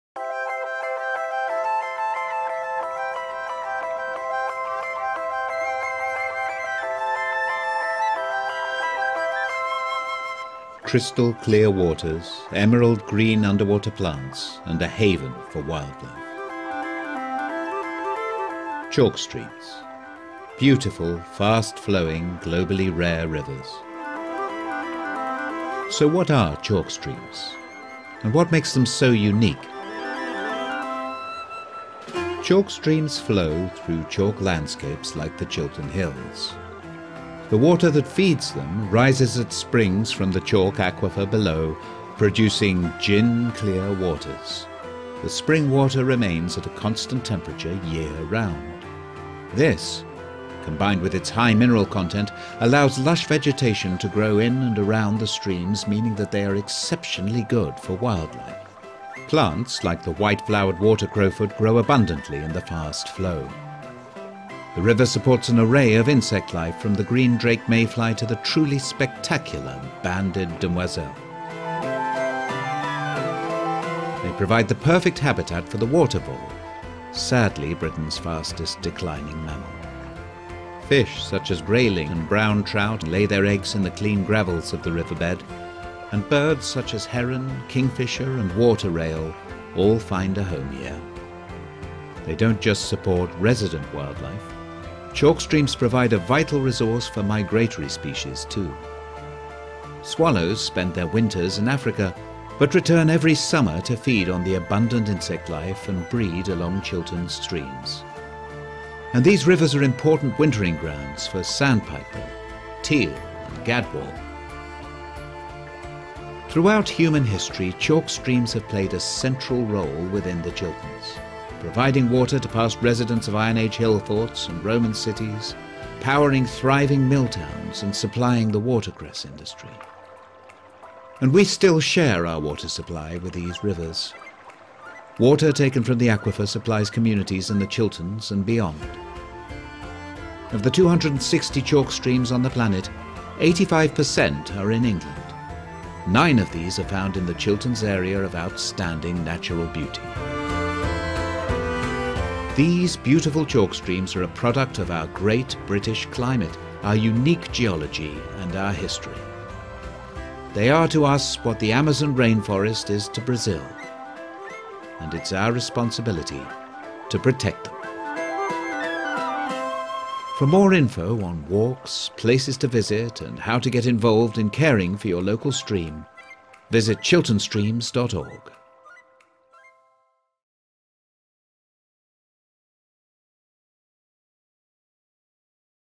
What is a chalk stream?
What is a chalk stream, how do they work, and what makes them so special?